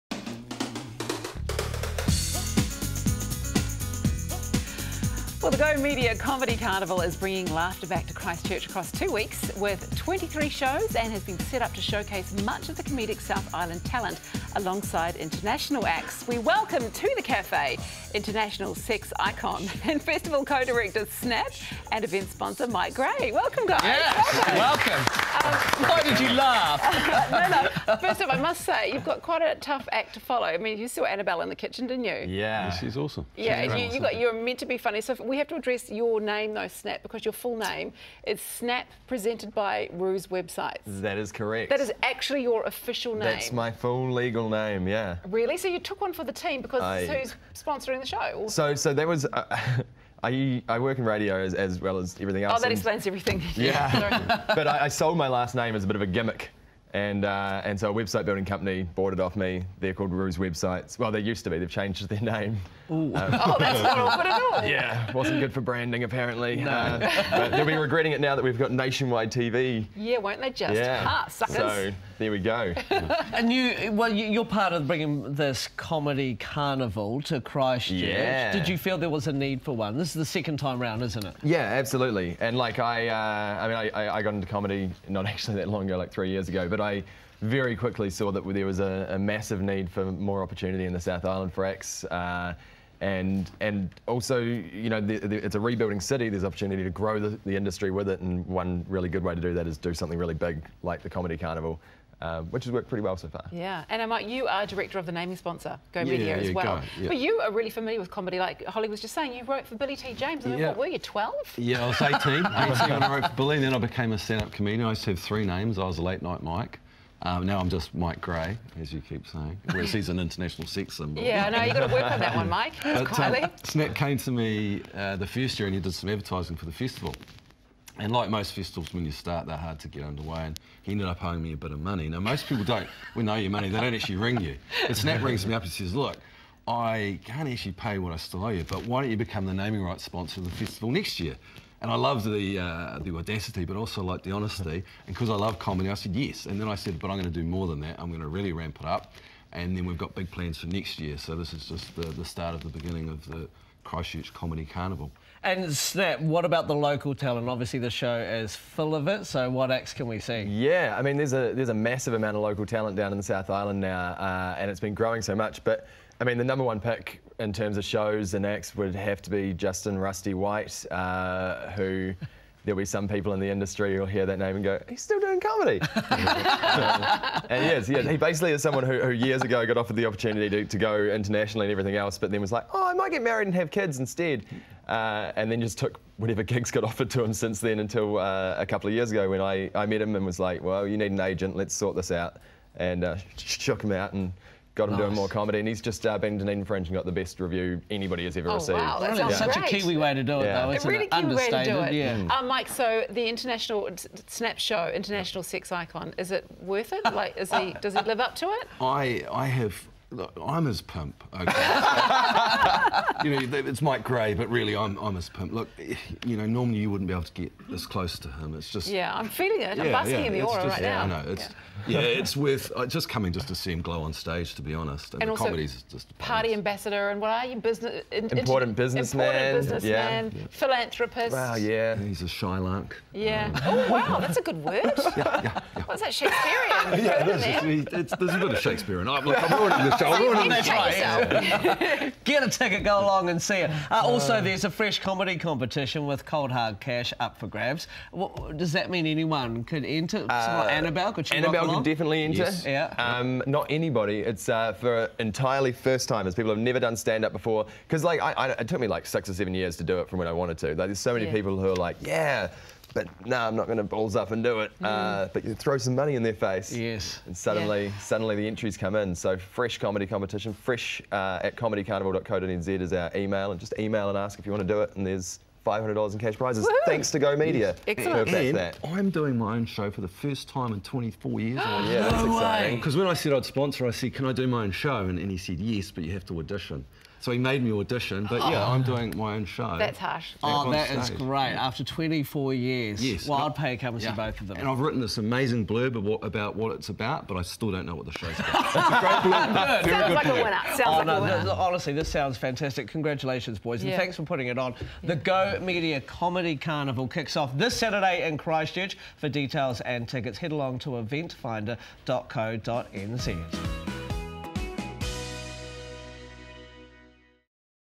Comedians Snap and Mike Gray Join Us
The Go Media Comedy Carnival is bringing laughter back to Christchurch across 2 weeks with 23 acts and has been set up to showcase much of the comedic South Island talent alongside international acts. This morning we were joined by International Sex Icon and festival Co-Director Snap and event sponsor Mike Gray!